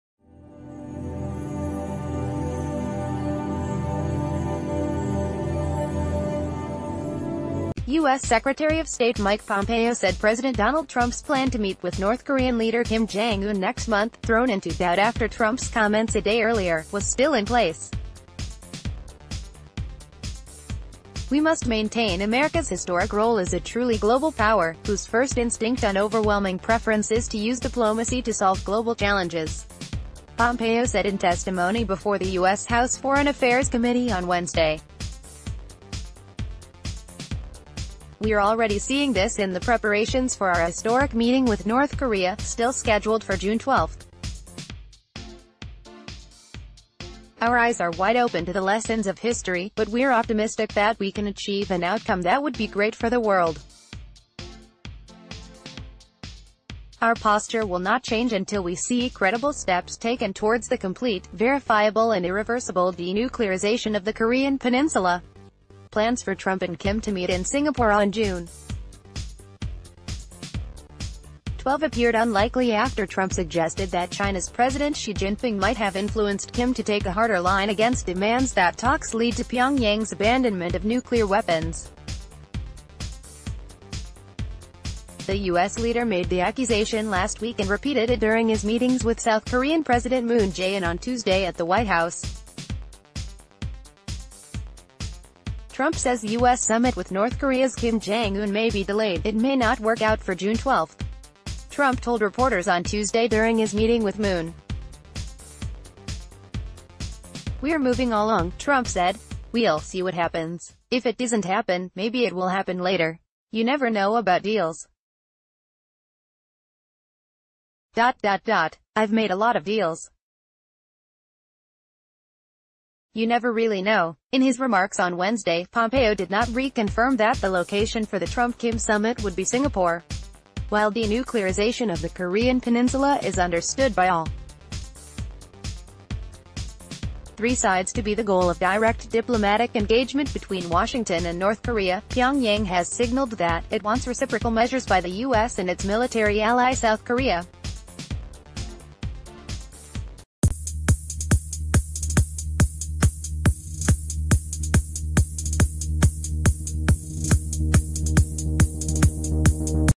Breaking News | US secretary of state says Trump’s summit with Kim is still on
US Secretary of State Mike Pompeo said President Donald Trump’s plan to meet with North Korean leader Kim Jong-un next month, thrown into doubt after Trump’s comments a day earlier, was still in place. “We must maintain America’s historic role as a truly global power, whose first instinct and overwhelming preference is to use diplomacy to solve global challenges,” Pompeo said in testimony before the US House Foreign Affairs Committee on Wednesday. “We’re already seeing this in the preparations f...